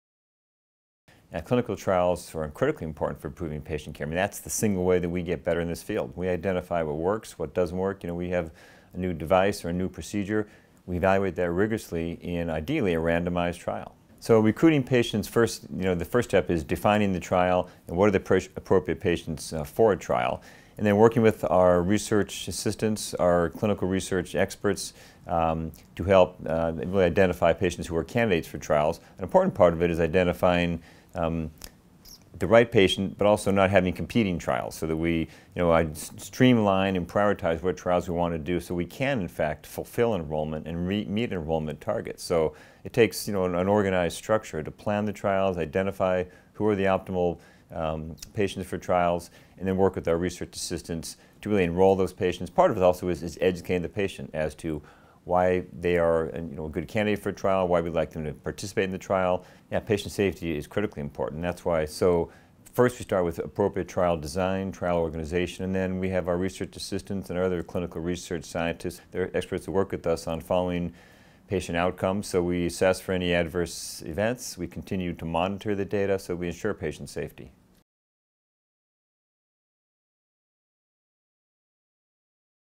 Why Are Clinical Trials Important? (HSS)
HSS has been a leader in advancing novel treatments through innovative clinical trials aimed at improving the lives of people with orthopedic, rheumatic, pain, and neurologic conditions.

Hear from Scott Rodeo, MD, Vice Chair of Orthopaedic Research, co-director of the Orthopaedic Soft Tissue Research Program, and director of the Center for Regenerative Research at HSS, about the importance of clinical trials, how they are designed and organized, and more.